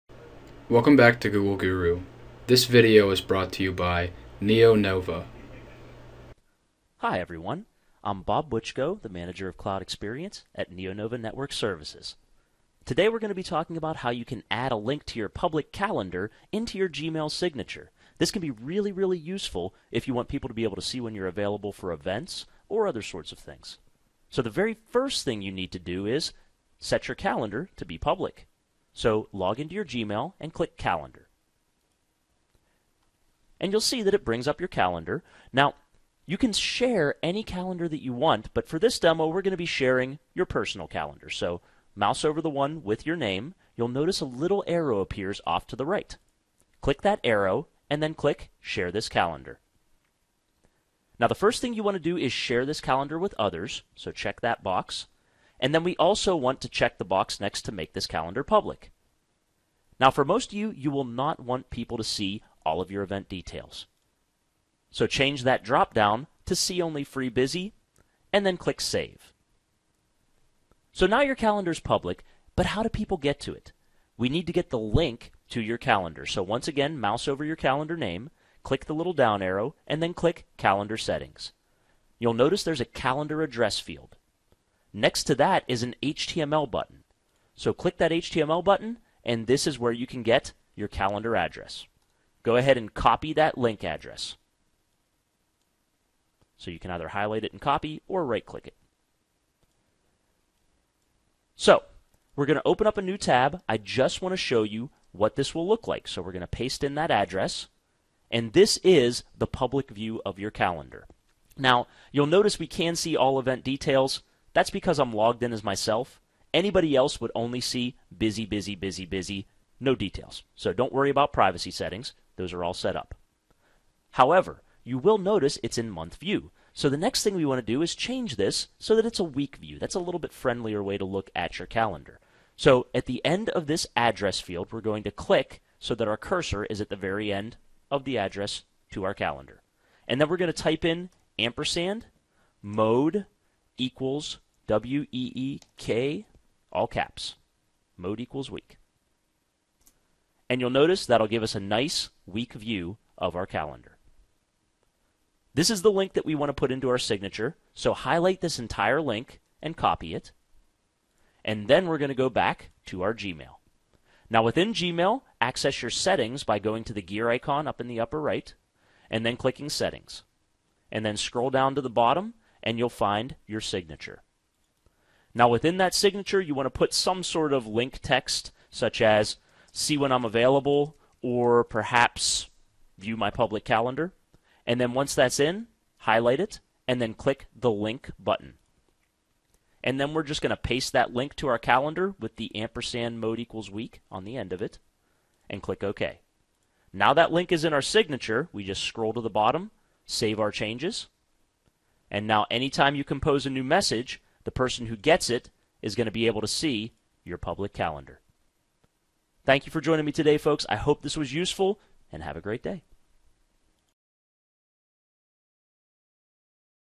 Add a link to your public Google Calendar in Gmail Signatures
Sharing your public Google Calendar is a great way to let your contacts know when you're available, but what if they're not sure where to look? Adding a link to your calendar in a Gmail signature is a great way to make this information readily available.

In this video we discuss how to set your Google Calendar to public, along with a great way to customize the view so it's set to 'Week' view, rather than 'Month'.

From there, it's as simple as adding a hyperlink with some descriptive text to your Gmail signature. If you're unsure how to do any of these tasks, be sure to check out the video for more information!